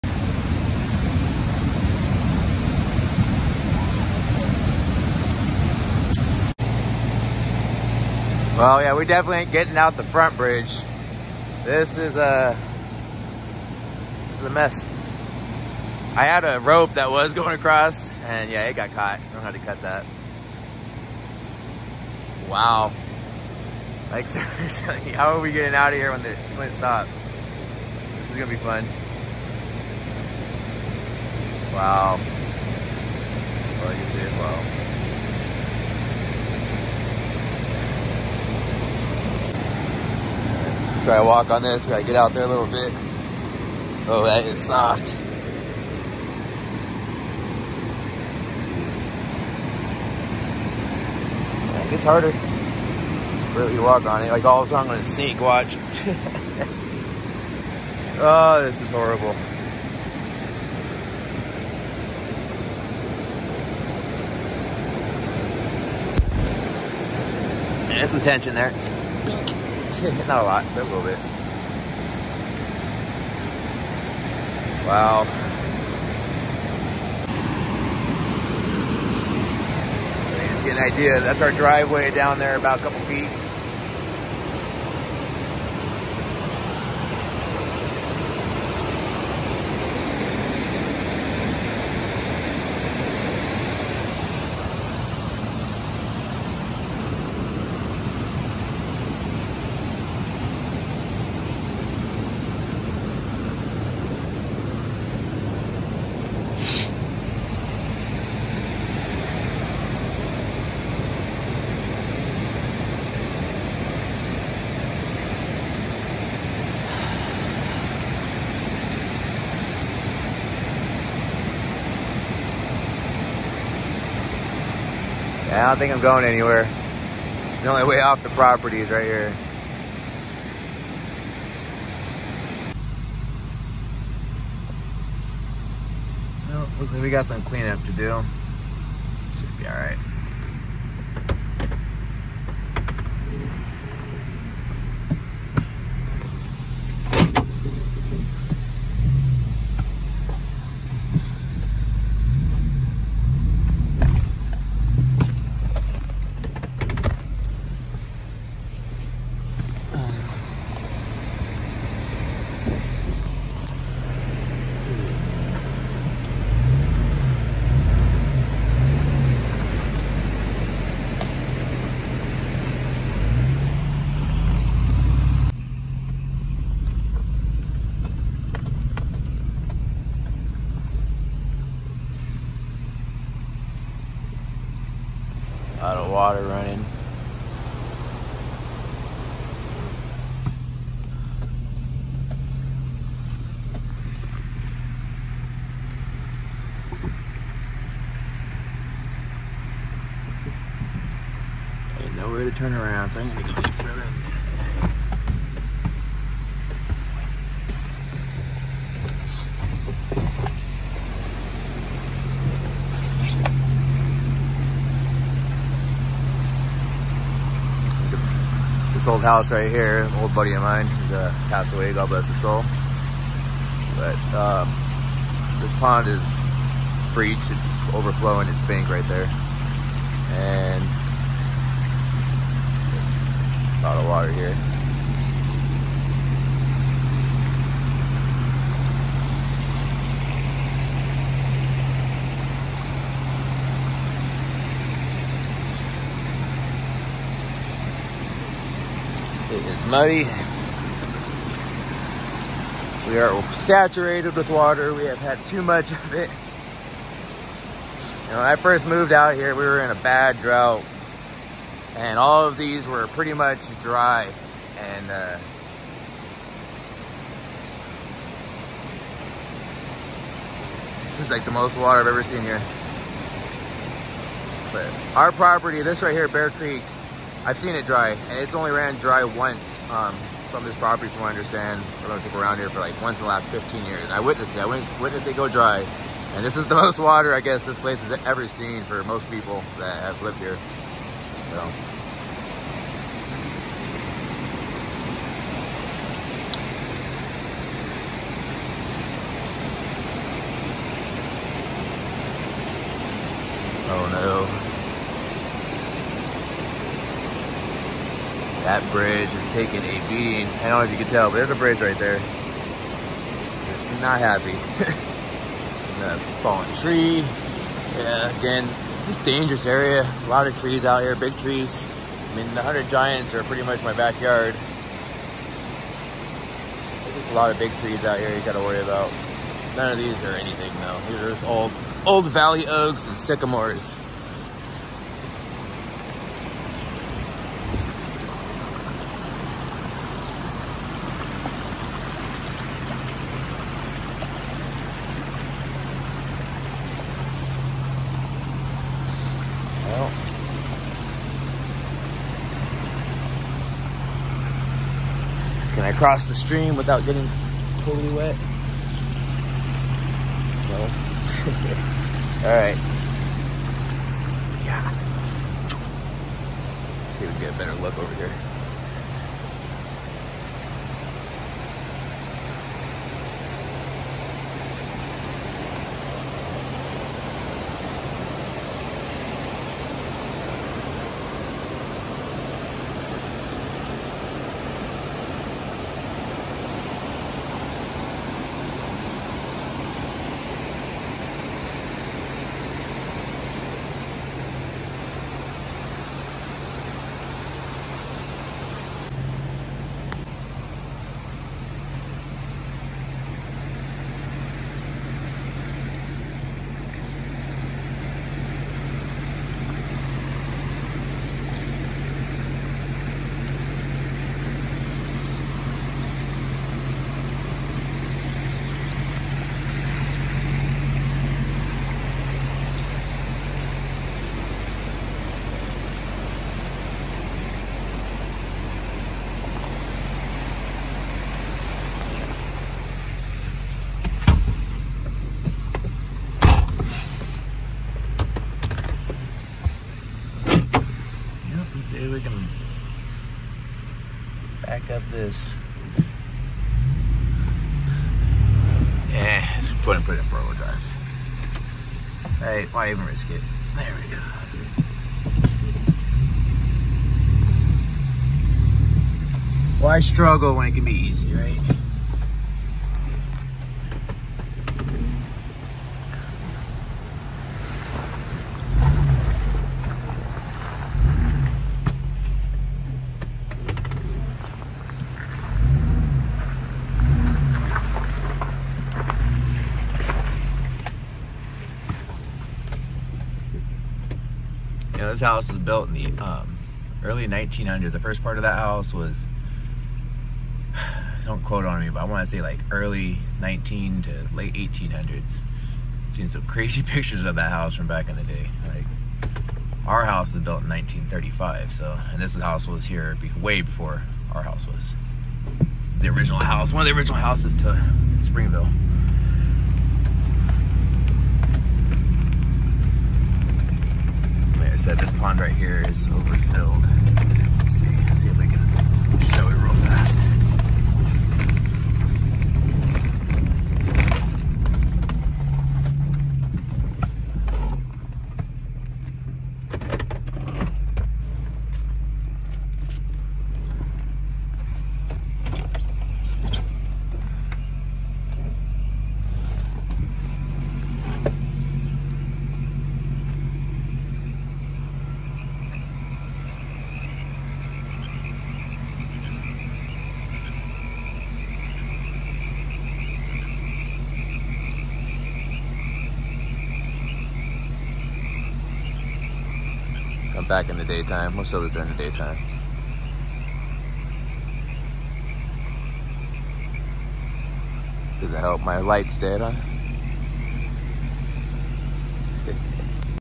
Snow Melt SURPRISE: What Shocking Effects has it had on California?
Another hard hit but this time its from the snow melt. This is the only way off the property other than a airlift. We are safe but we didnt expect this to be this bad CA snow melting flooding springville #2023

ca snow melting california storm springville california atmospheric river california